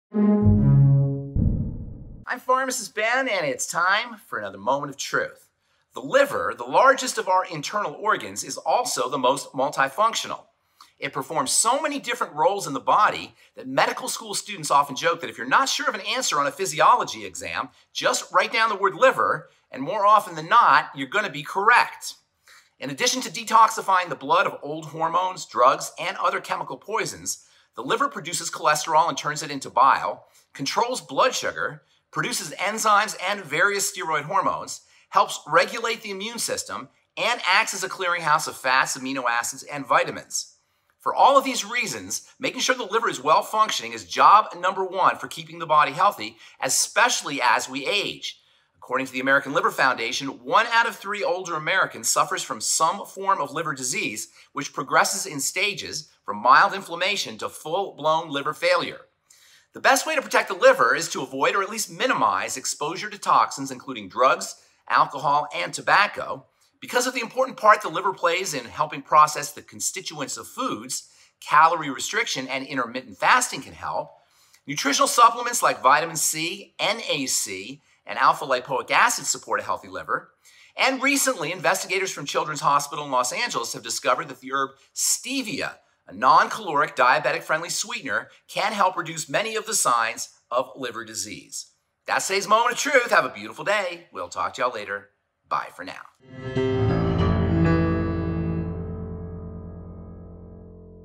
Liver Love - Pharmacist Ben Fuchs - Moment of Truth
The liver the largest of our internal organs, is also the most multi-functional.  It performs so many different roles in the body that medical school students often joke that, if you're not sure of an answer on a physiology exam, just write down the word liver and more often than not you're going to be correct.  In addition to detoxifying the blood of old hormones, drugs and other chemical poisons, the liver produces cholesterol and turns it into bile, controls blood sugar, produces enzymes and various steroid hormones, helps regulate the immune system and acts as a clearinghouse of fast amino acids and vitamins.

For all of these reasons, making sure the liver is well functioning is job number one for keeping the body healthy, especially as we age.  According to the American liver foundation, one out of three older Americans suffers from some form of liver disease, which progresses in stages from mild inflammation to full-blown liver failure.  The best way to protect the liver is to avoid or at least minimize exposure to toxins including drugs, alcohol and tobacco.

Because of the important part the liver plays in helping process the constituents of foods, calorie restriction and intermittent fasting can help.  Nutritional supplements like vitamin C, NAC and alpha lipoic acid support a healthy liver.  Recently, investigators from Children's Hospital in Los Angeles have discovered that the herb Stevia, a non-caloric diabetic friendly sweetener can help reduce many of the signs of liver disease.